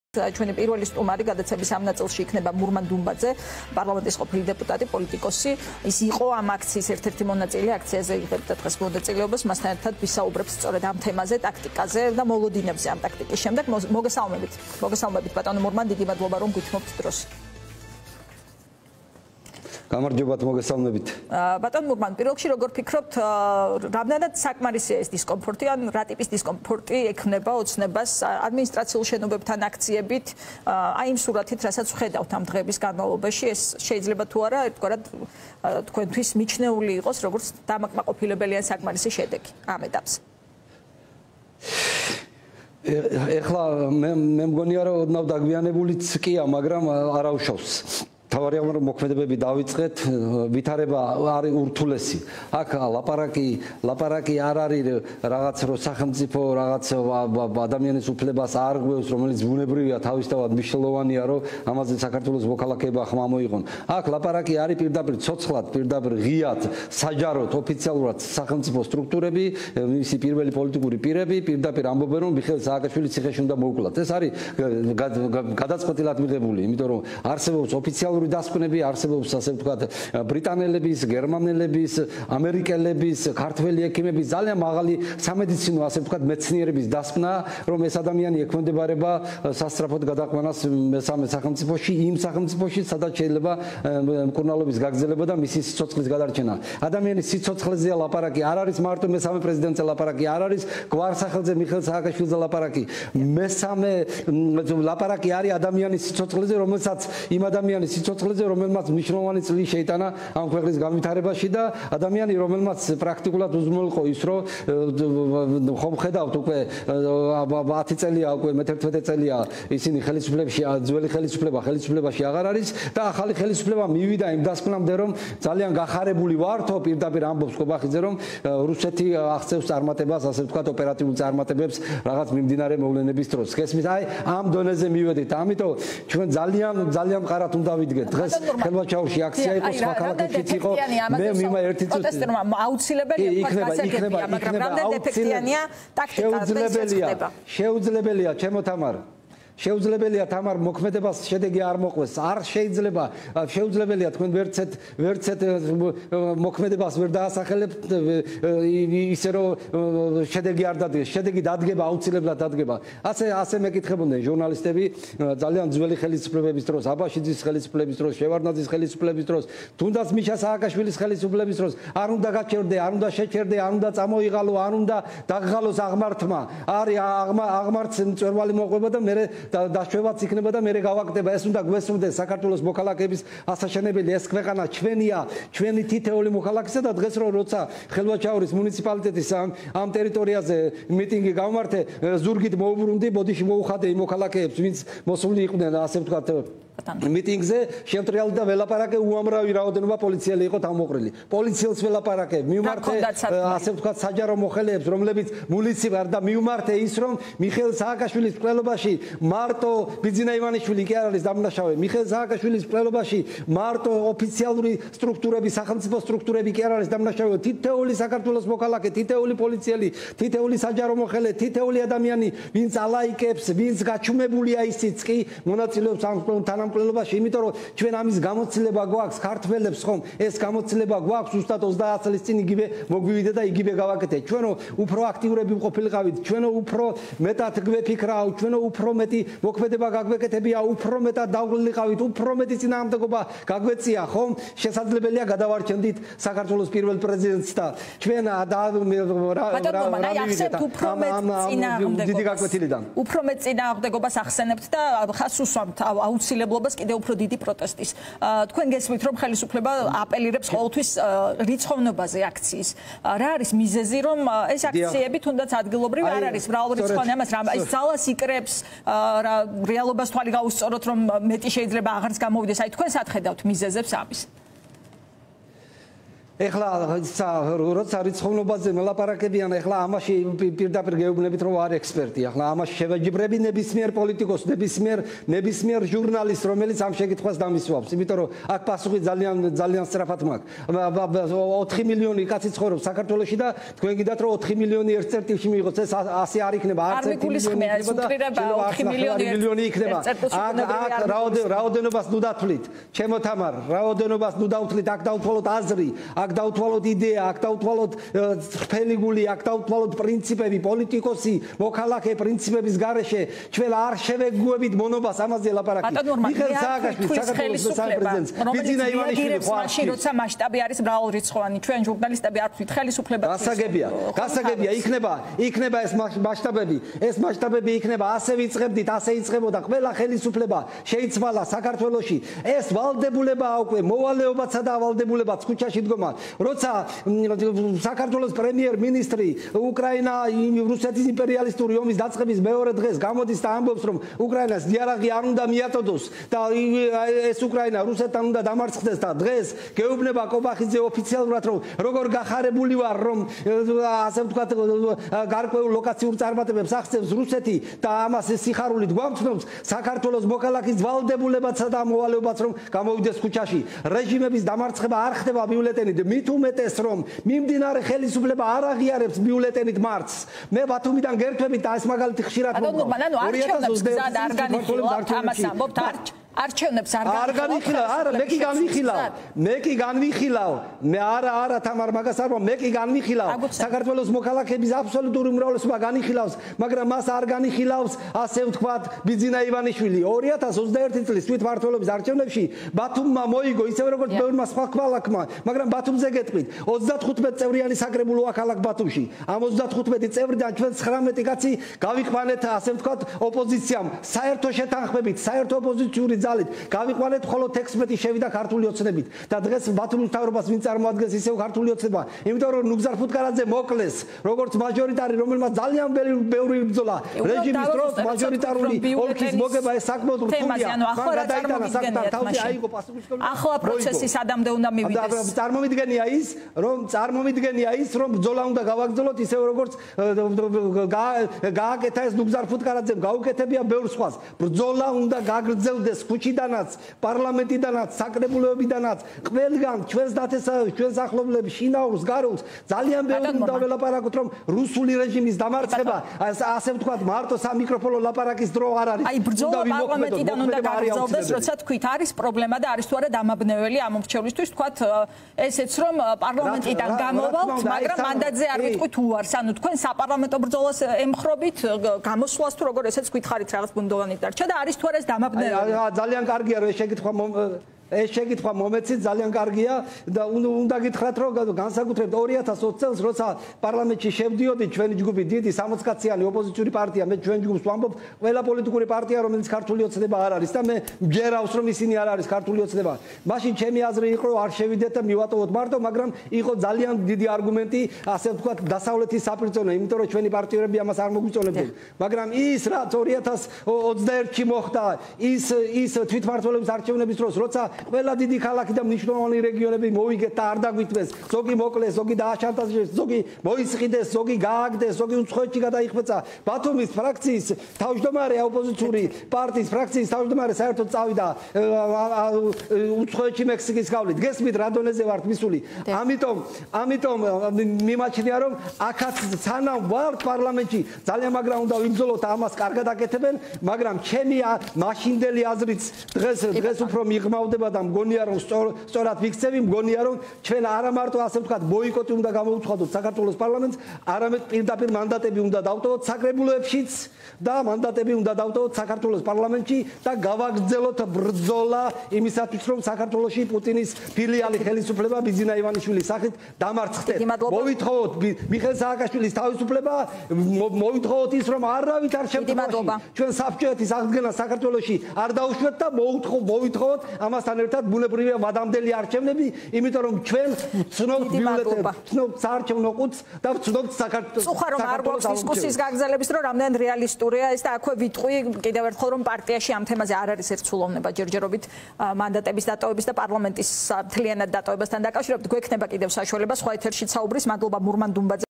მურმან დუმბაძე | კომენტარი
▪️ „სააკაშვილის მკვლელობაში მარტო ივანიშვილი და სახელმწიფო სტრუქტურები არ არის დამნაშავე, თითოეული საქართველოს მოქალაქე, ვინც გაჩუმებულია, მონაწილეობს ამ მკვლელობაში“ - მურმან დუმბაძე